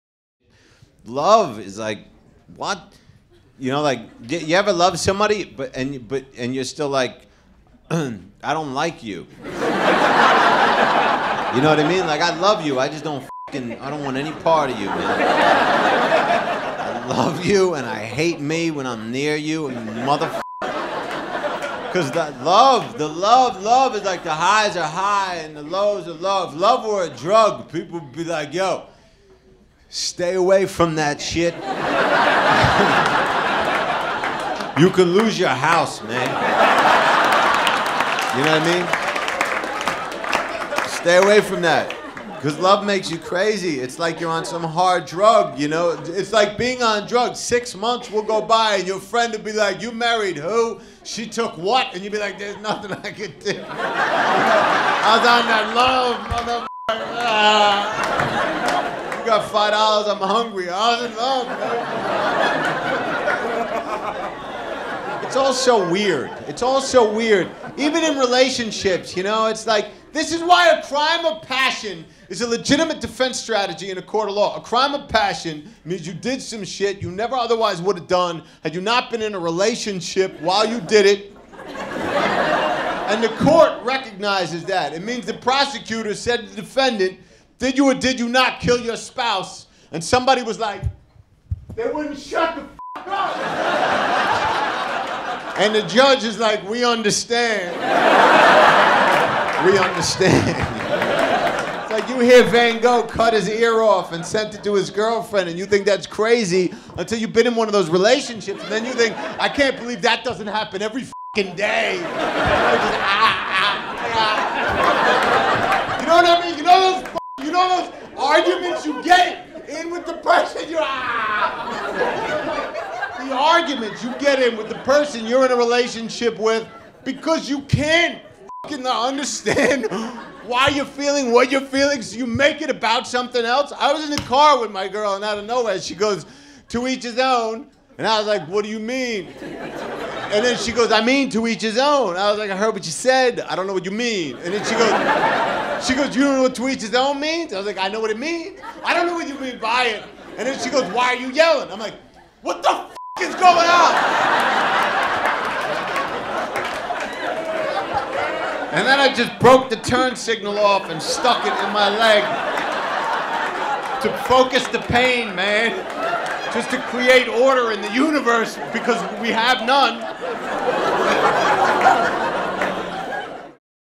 Dov Davidoff - Love Drug
Dov Davidoff (@dovdavidoff) focuses the pain of being in a relationship at the Laugh Factory comedy club in Hollywood, CA, home of the best stand up comedians.